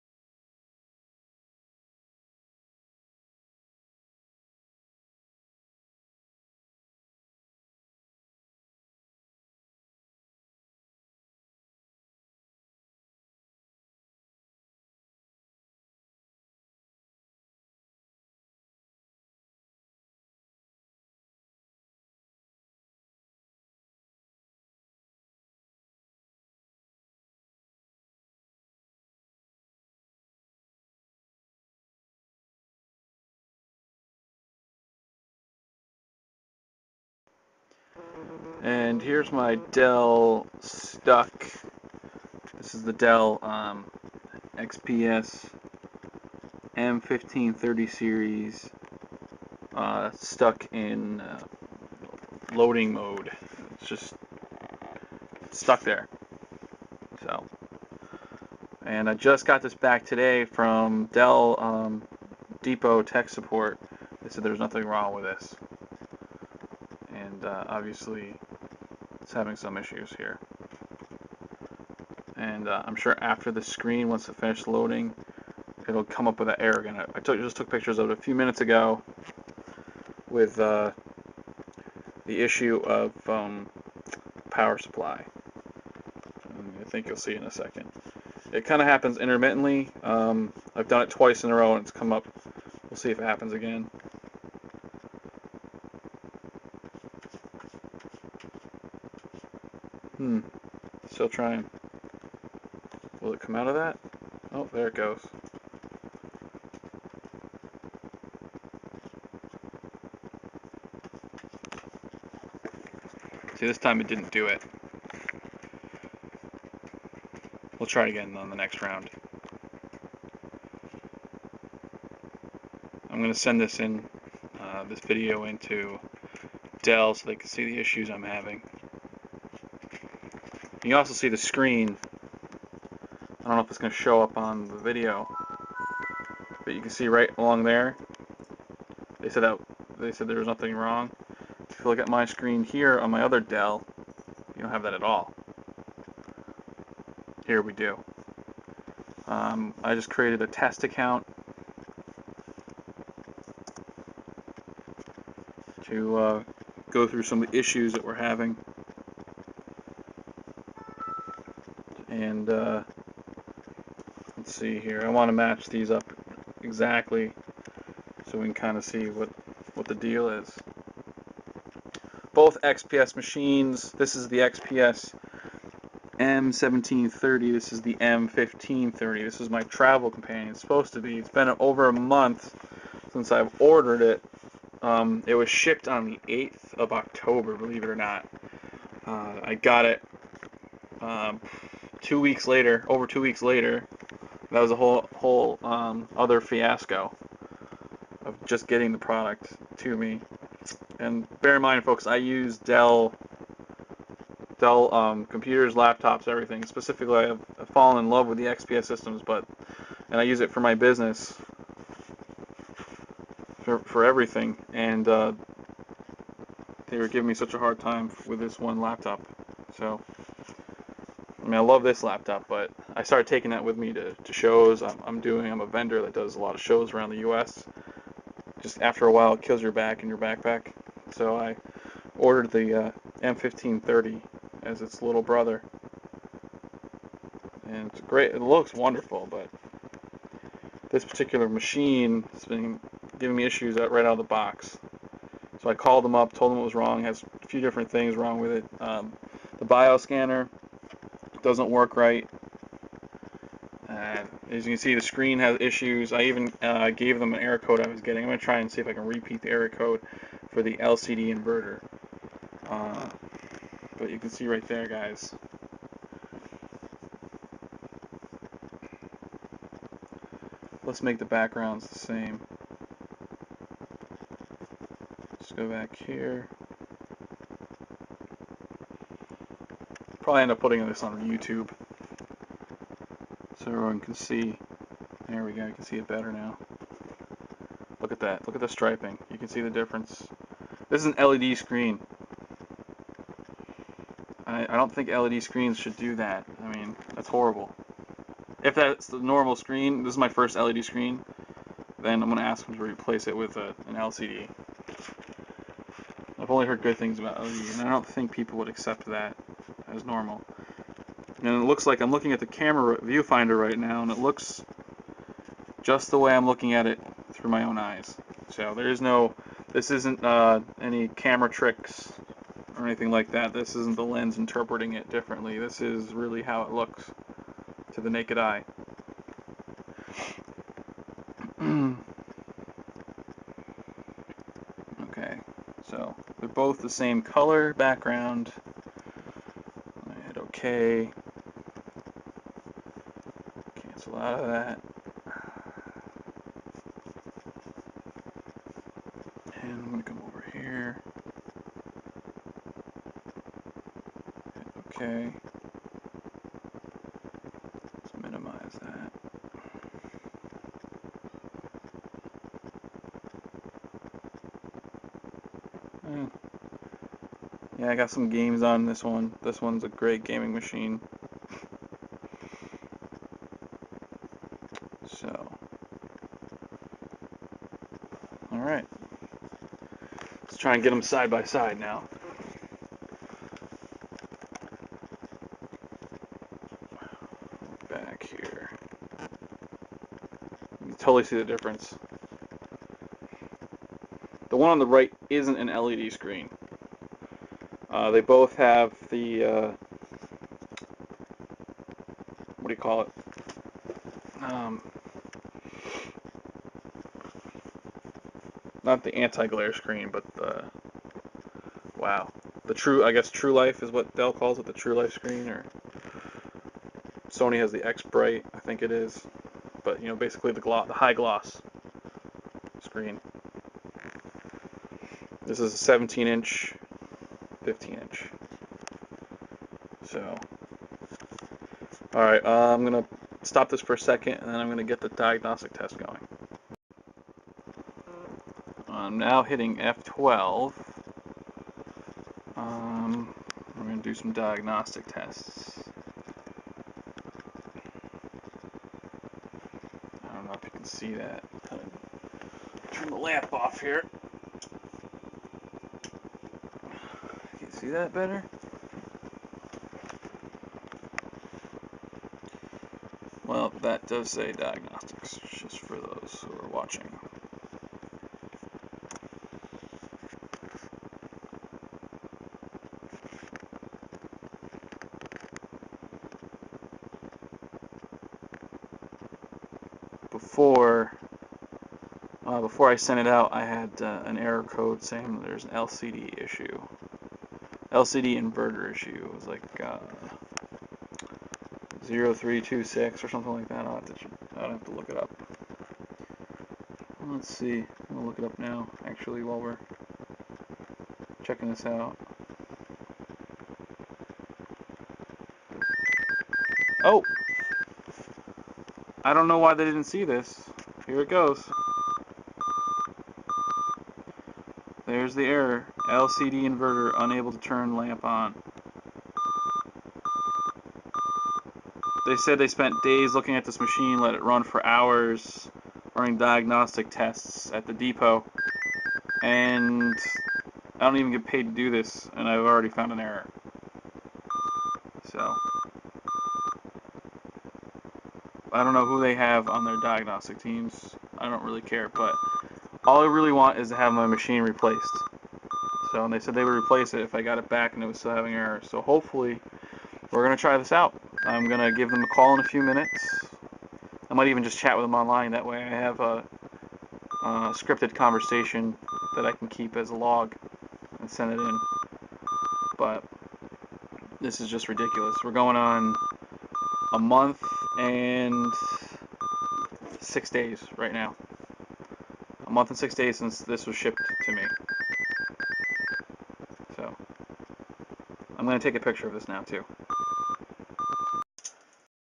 Dell XPS M1530 Hardware Issues And Diagnostic Testing Part 1
Part 1

This is a diagnostics test video log recording all the problems with my new/refurbished Dell XPS M1530 laptop. Hopefully this video will aid in replacing my laptop now that it is out for the world to see. Here is the background to my story:

I Waited weeks for my Dell XPS M1530 (Purchased it from Dell Outlet.) When I got it I noticed a few things that weren't right. I Called and troubleshot over the phone and online through Dell chat. I eventually got it sent to the Dell Repair Depot but their techs could find nothing wrong after "Days of Testing". I ran some diagnostic tests the day I got it back from them. This was a month and six days later from the day it was shipped from Dell. After just a few minutes of testing I got an error for the LCD inverter. I also received a power error upon booting up and I have backlight issues for my LED's. I also have a problem with the bio-scanner. All of this has been documented on this short video. To top it off it looks like my screen had someone's greasy face smeared across it and then a little "wax on wax off" action applied to it. There were a bunch of scratched near the top of the keyboard bezel as well.

If anyone else has had issues like this I would love to hear from you. Maybe then we can get something accomplished together. I usually buy my computers from the Dell Outlet and have never had any issues from them. When I received this one I called and complained but they didn't want to be bothered with replacing it. Oh well, well see about that now.

Let me know if you have had similar issues with Dell relating to receiving a broken product.